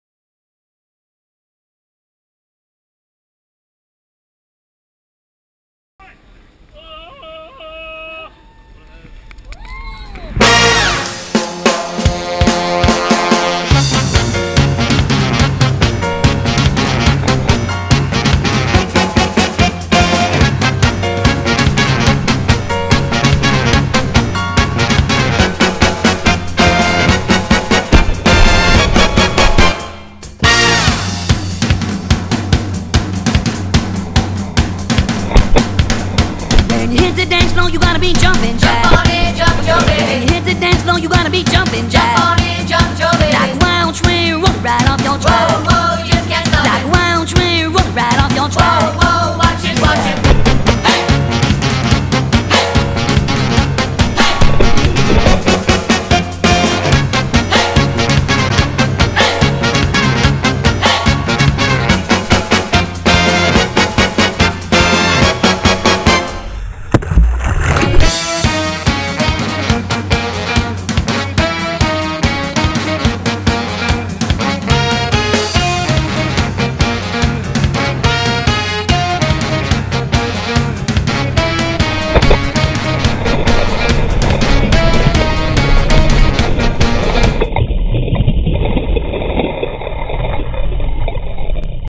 Manly's Jumpin - Tombstoning at Jump Rock Manly - Shot on the GoPro HERO2 at 100fps Slow Motion
Manly's Jumpin
Tombstoning, Rock jumping and some great Backflips and who dives from handstand at 8 meters??? Well Sean the Madman does. Jump Rock Manly Sydney Australia
Shot on the GoPro HERO2 at 100fps Slow Motion